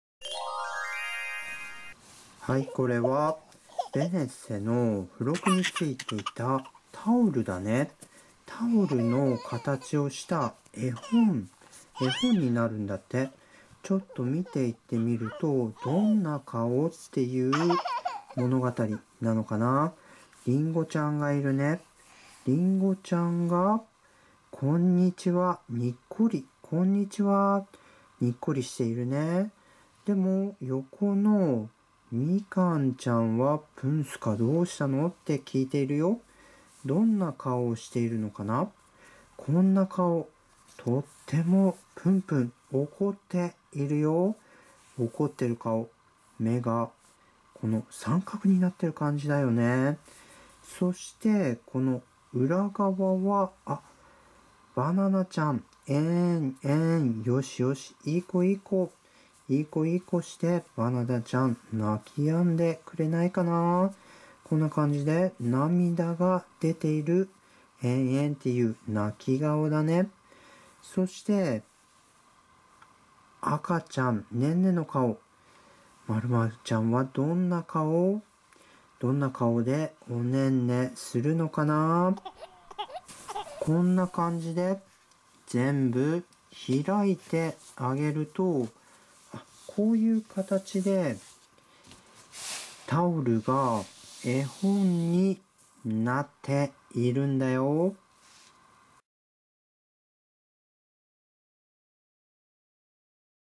ベネッセ エデュトイ ふろく えほんになるタオル BENESSE
It's a toy's clip おもちゃの動画で"りんご、オレンジ、バナナがどんなかおをするのかな？いっしょにおねんねしようね"。

おもしろかったら、シェアよろしくね(^_-)-☆

■つぎはこれをみようかな？

●ままごとトントン おりょうりおもちゃセット